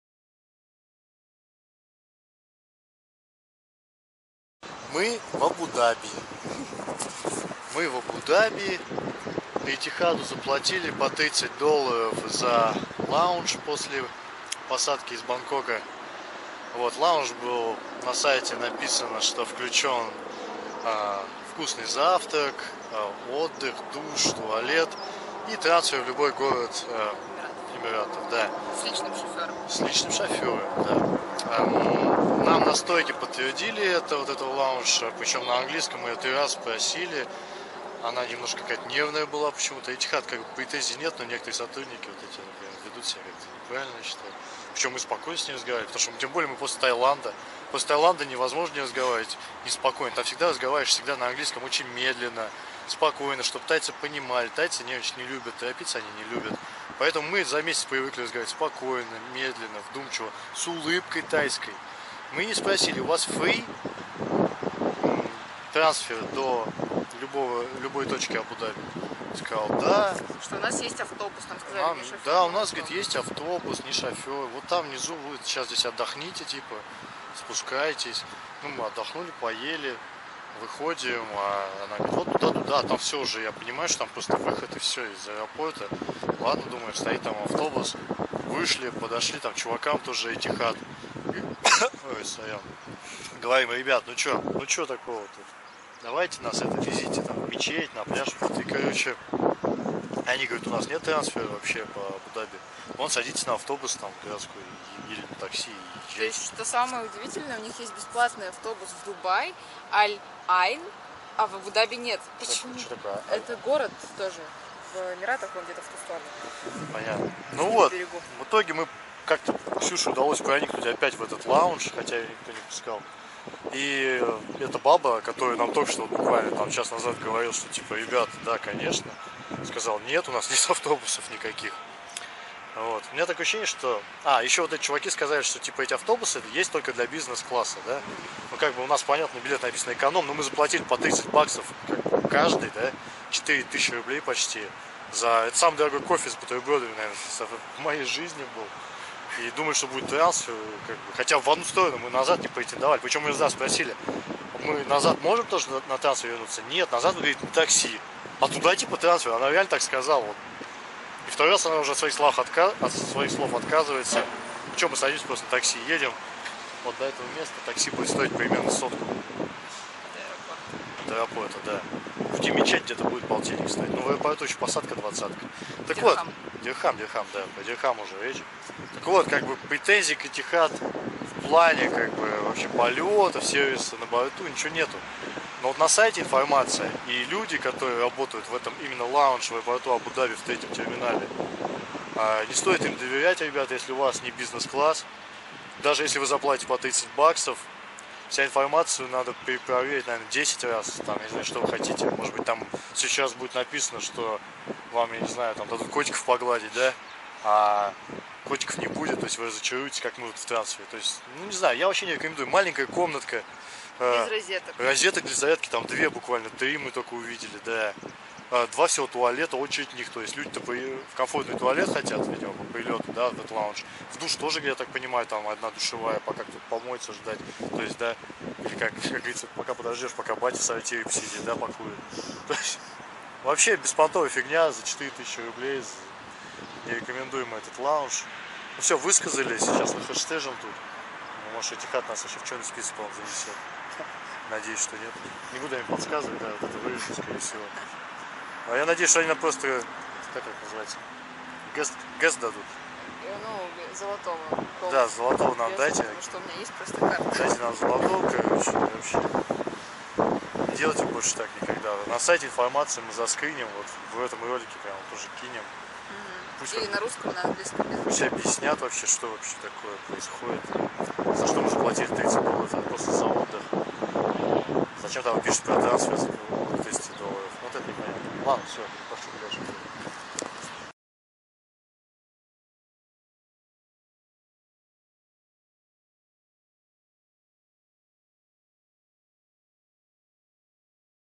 Наши вопросы к Etihad по поводу Arrivals lounge в Абу-Даби.
Заплатили 30$ с человека, ждали обещанный трансфер,а поехали в итоге на такси...